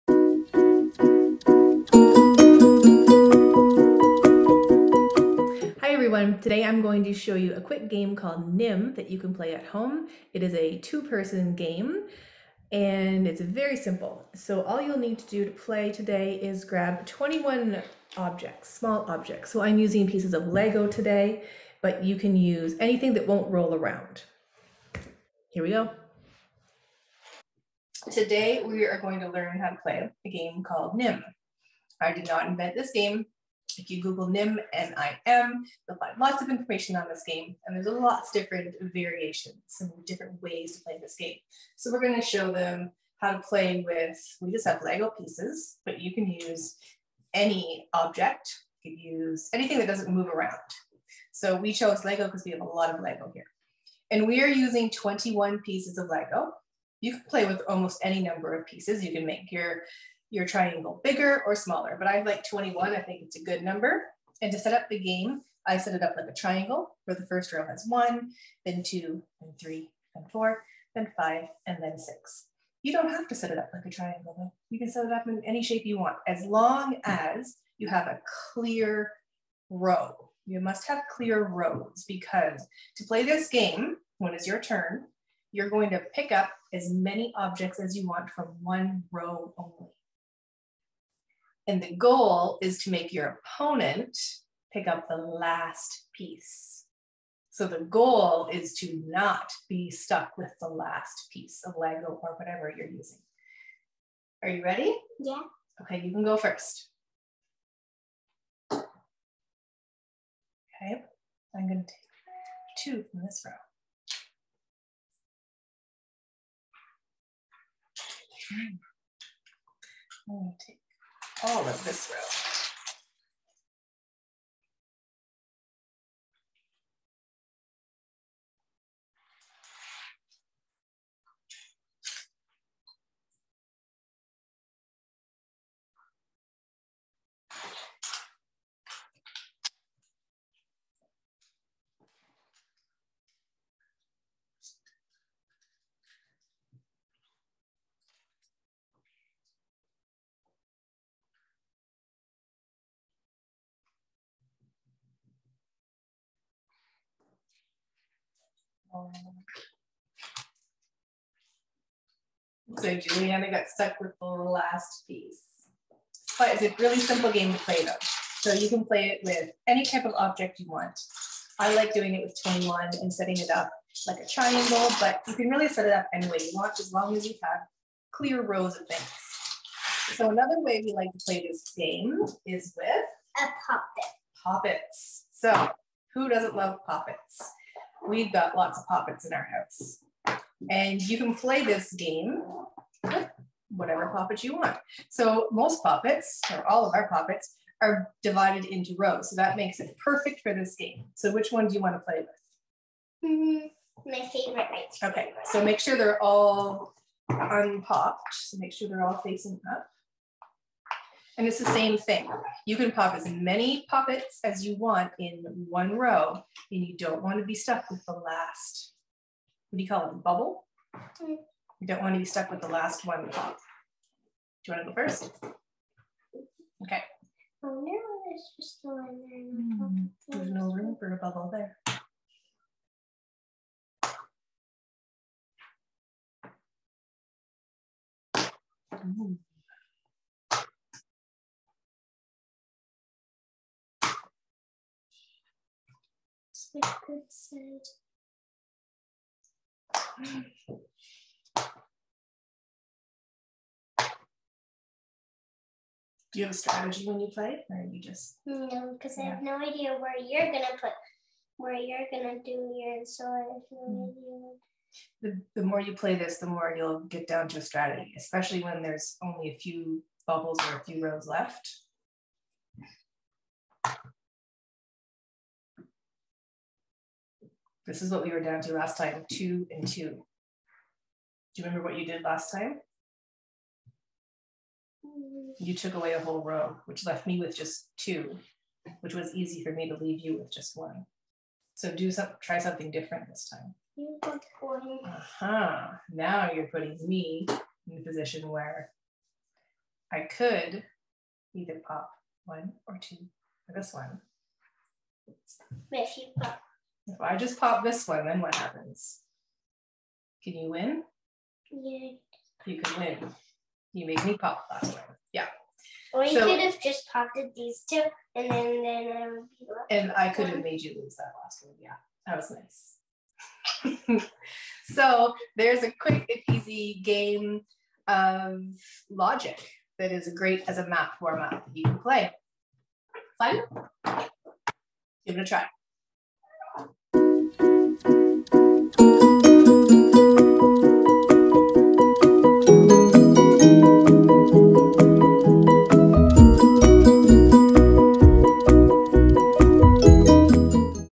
NIM LOGIC GAME | Logic game with a pop it | Simple math game you can play at home
In this video, I will give you step by step instructions on how to play a simple math game called Nim.  You can play this logic game anywhere and you can even play it with your favourite pop-it!